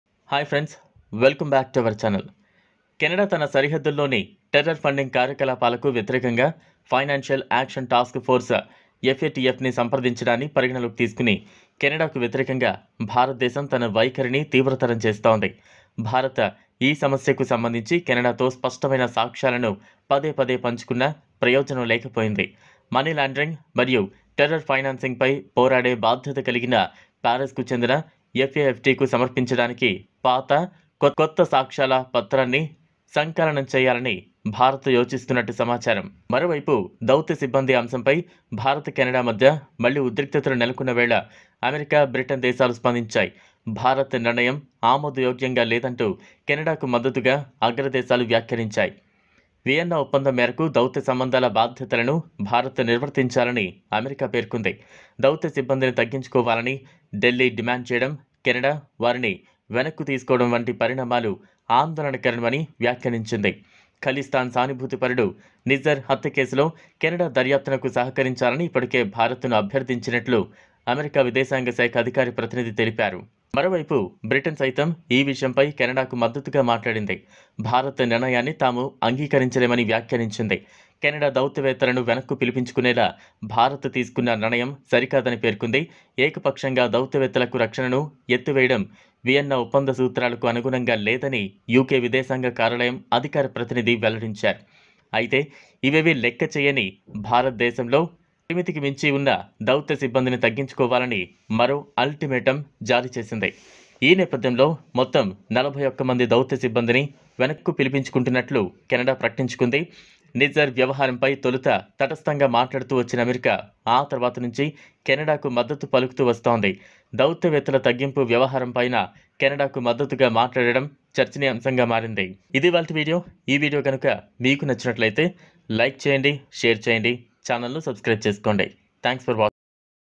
48 గంటల్లో కెనడాకు చేసిన డబల్ ధమాకా.! దెబ్బకి అతి పెద్ద గట్టి వార్నింగ్ ఇచ్చిన భారత్ || SFI
Watch: 48 గంటల్లో కెనడాకు చేసిన డబల్ ధమాకా.! దెబ్బకి అతి పెద్ద గట్టి వార్నింగ్ ఇచ్చిన భారత్ || SFI